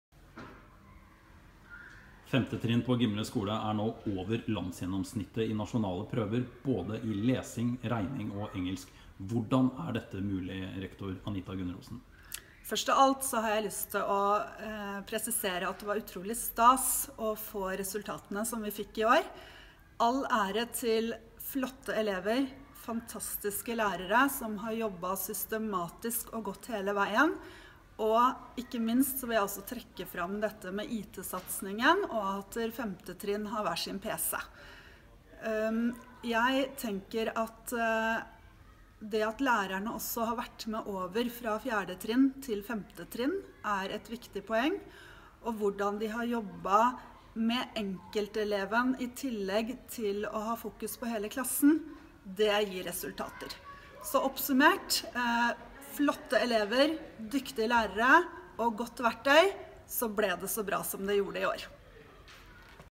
Flinke lærere og riktig verktøy gir gode resultater
Femte trinn ved Gimle skole i Halden kommune gjør det bedre enn landsgjennomsnittet nasjonale prøver i både lesing, regning og engelsk. I dette intervjuet fra januar 2018 forklarer rektor Anita Gundrosen hvorfor.